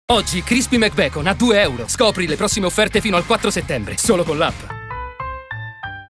Sono arrivati i Crispy Days: oggi Crispy McBacon® a 2€!
Crispy McBacon® compie 20 anni. E c’è più gusto a festeggiarli insieme.
Come? Con i Crispy Days! Fino al 4 settembre, 5 giorni di offerte eccezionali
all’insegna di Crispy McBacon® e di tutte le varianti.